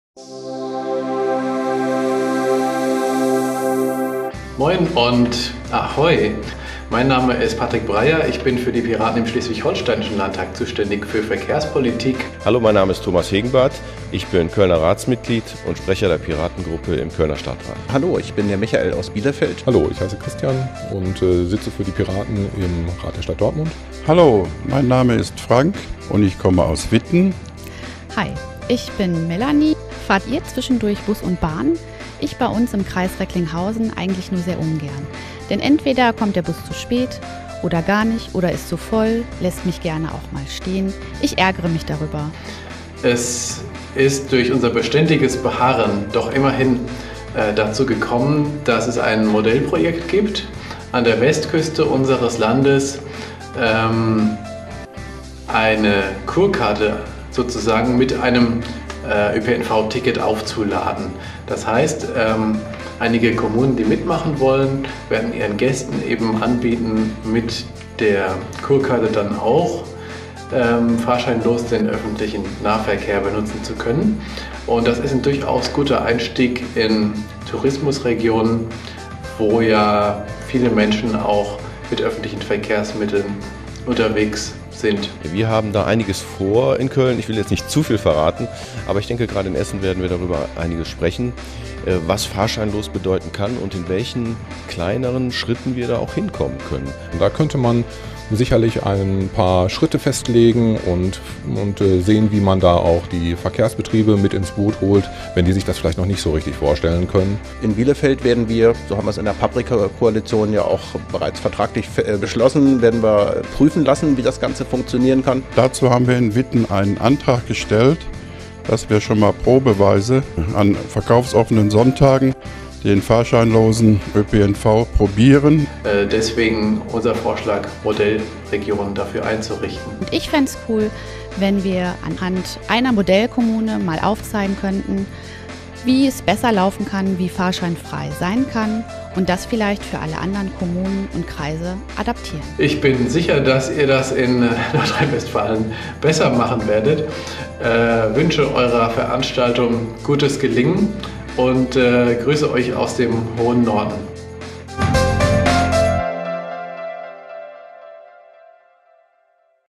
Diese Piraten fahren am 12.03.2016 nach Essen zu
VERANSTALTUNG
Modellkommune Fahrscheinfrei
am Samstag 12. März 2016 / 11 – 17.30 Uhr
im Unperfekthaus Essen

Save the Date: Auftakt- und Informationsveranstaltung zur NRW-Modellkommune

Eine Veranstaltung für alle, die mehr aus ihrem ÖPNV vor Ort machen wollen:

Bus & Bahn fahrscheinfrei • Bürgerticket • Solidarische Umlagefinanzierung

Wie etabliere ich in meiner Stadt, Region oder Gemeinde einen attraktiveren öffentlichen Nahverkehr?
Wie ebne ich vor Ort den Weg zu einem Modellprojekt „Bus & Bahn fahrscheinfrei“?

Wir suchen die NRW-Modellkommune fahrscheinfrei! Hierfür wollen wir uns als Vorbild und Praxisbeispiel im Sommer 2016 einem Ort besonders widmen und dort zeigen, wie sich ein Modellprojekt „Bus und Bahn fahrscheinfrei“ verwirklichen lässt. Auf der Auftaktveranstaltung – dem fahrscheinfreitag – informieren die Piratenfraktion im Landtag NRW und die PiKo NRW – Piraten in der Kommunalpolitik in NRW e.V. gemeinsam über die Initiative NRW fahrscheinfrei.

Auf dem fahrscheinfreitag erweitern wir unser Wissen über ÖPNV und die Situation in den Kommunen. Wir suchen gemeinsam Orte, die sich für ein Modellprojekt eignen und bereiten maßgeschneiderte Initiativen vor.

Save the Date: erstmals am Samstag, 12. März 2016, 11 bis 17.30 Uhr, im Unperfekthaus (UPH), Friedrich-Ebert-Straße 18, 45127 Essen

Die Teilnahme ist kostenfrei, die Verpflegung ist inbegriffen.

Anmeldung ab dem 1.